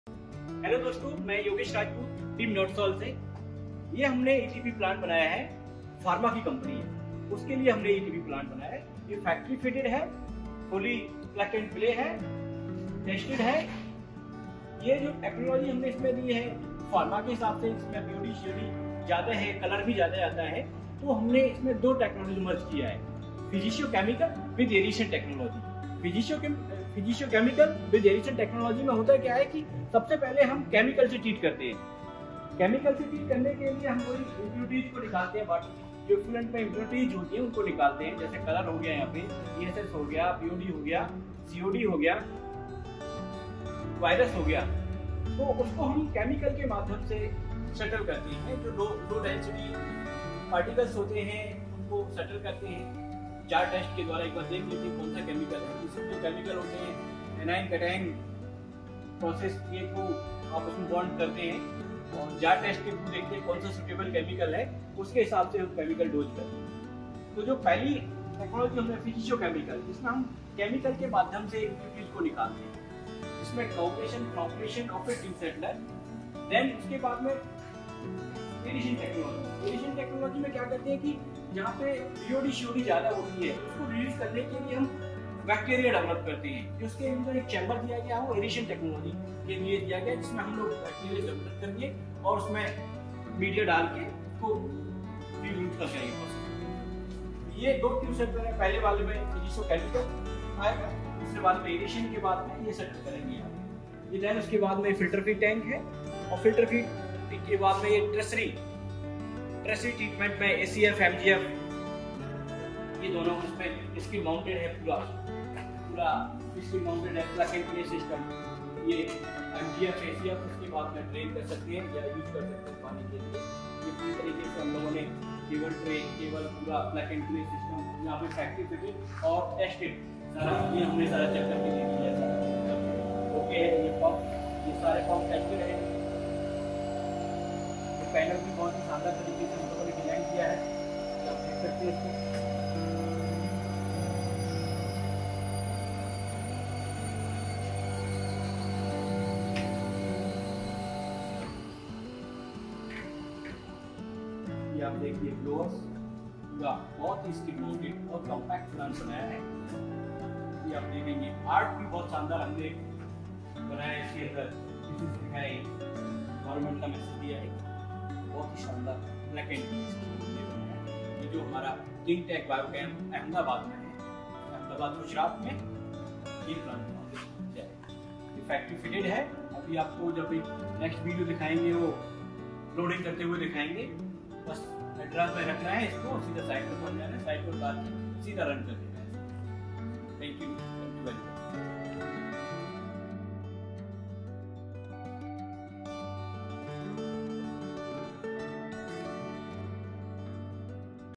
Factory Fitted ETP Plant for Pharma & Hospitals | Plug & Play ETP plants
Factory Fitted ETP Plant for Pharma & Hospitals | Plug & Play ETP plants for hospital and pharma industry. Removes all the toxins, color and other solvents from wastewater as per Pollution control board norms.

Watch NetsolWater's advanced factory-fitted effluent treatment plants (ETPs) designed specifically for pharmaceutical companies and hospitals. Our plug-and-play ETPs offer a complete, automated solution for efficient wastewater treatment.

🎁 Benefits of ETPs in Pharma & Hospital Industries:

👉 Environmental Compliance: Ensure adherence to stringent environmental regulations and reduce pollution.
👉 Public Health: Protect public health by effectively treating wastewater containing potentially harmful contaminants.
👉 Resource Conservation: Reduce water consumption through wastewater reuse and recycling.
👉 Cost Savings: Optimize operational efficiency and minimize treatment costs.

🧩 Key Features of Netsol Water's ETPs:

✔️  Factory-Fitted Design: Streamlined installation and reduced on-site construction.
Automation: Minimize manual intervention and ensure consistent performance.
✔️  Plug-and-Play Functionality: Easy setup and operation.
✔️  Customizable Solutions: Tailored to meet specific industry requirements and wastewater characteristics.

Watch this video to learn more about how Netsol Water's ETPs can elevate your pharmaceutical or hospital operations.